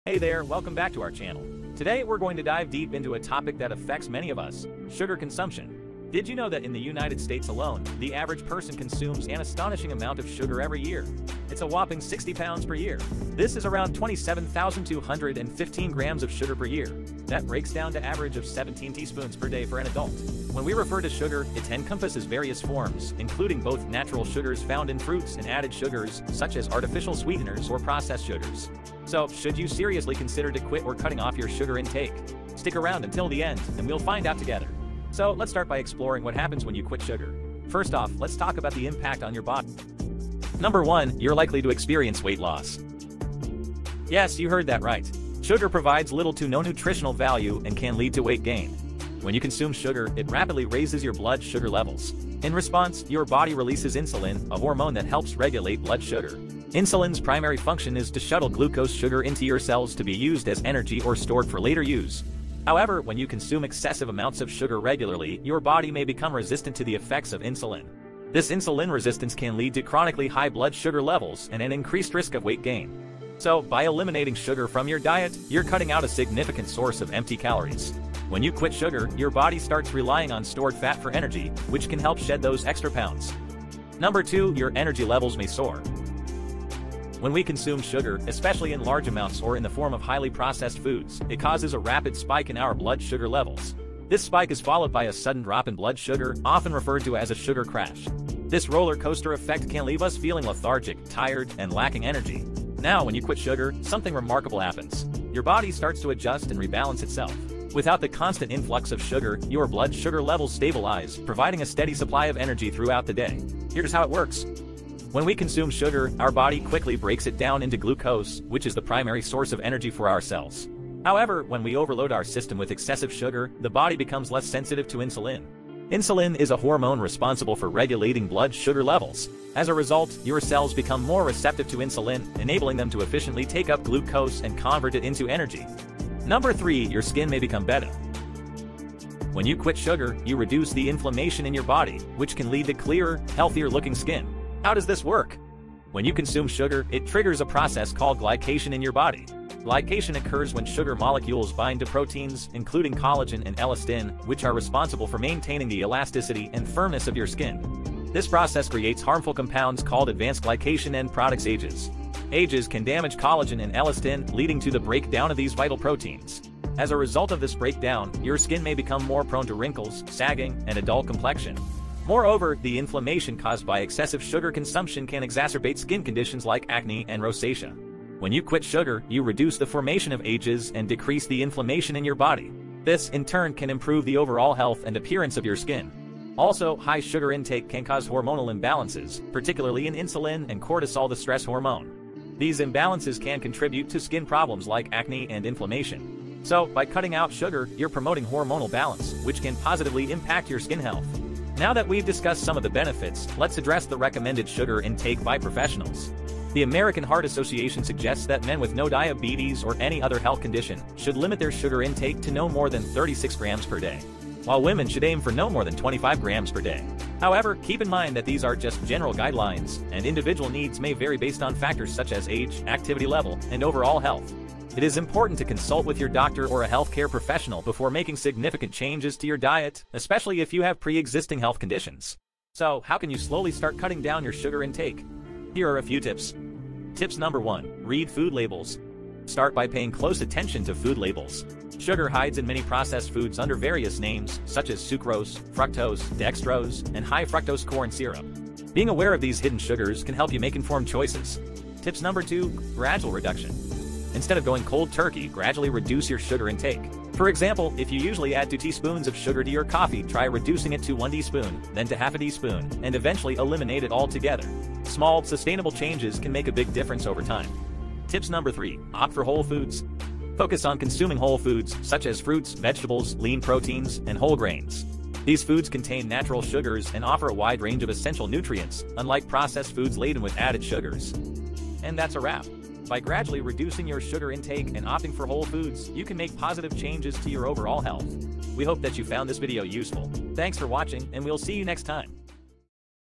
What Happens to your Body When You Quit Sugar?
Did you know that in the United States alone, the average person consumes an astonishing amount of sugar every year?

Today, we're going to dive deep into a topic that affects many of us: sugar consumption and what happens to your body when you quit sugar?

Join our supportive community as we empower you with knowledge, inspiration, and actionable steps towards achieving your wellness goals.